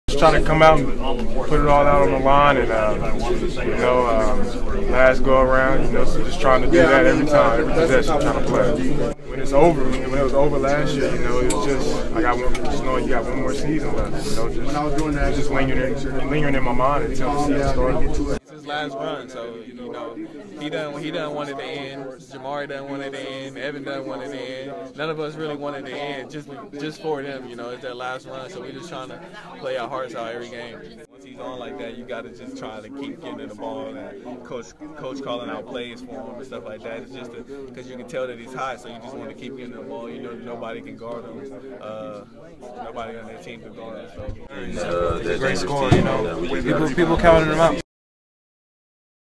Perry Ellis tops his NCAA Tourney high with 27 points
KU's Perry Ellis and his teammates don't want his final college season to end. Ellis did his part with 27 points Thursday as the Jayhawks advanced to the Elite Eight with a 79-63 win over Maryland.